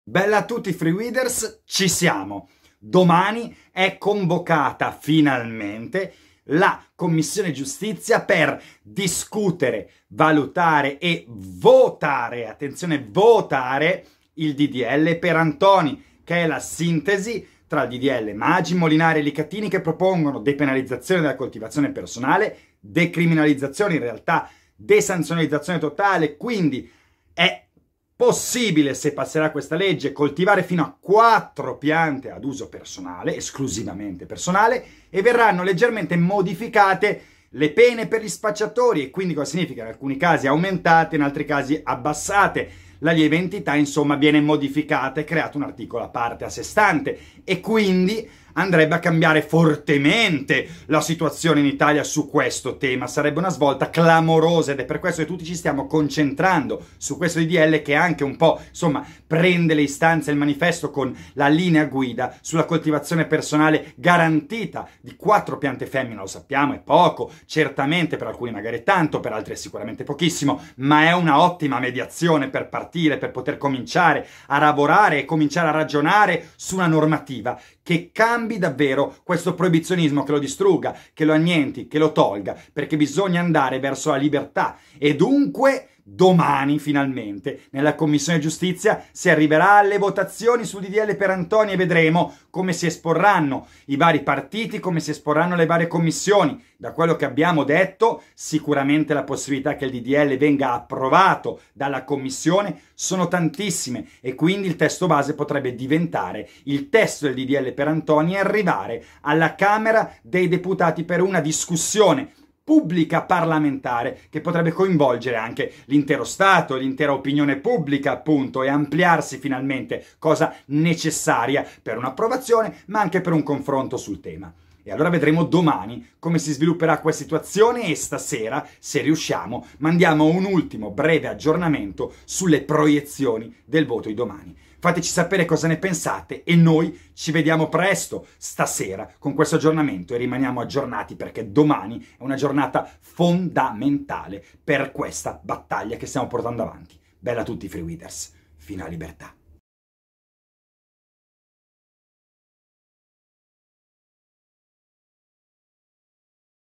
DOMANI SI VA AL VOTO IN COMMISSIONE GIUSTIZIA SUL DDL PERANTONI! FORSE CI SIAMO!
📰 Notiziario Giornaliero!📰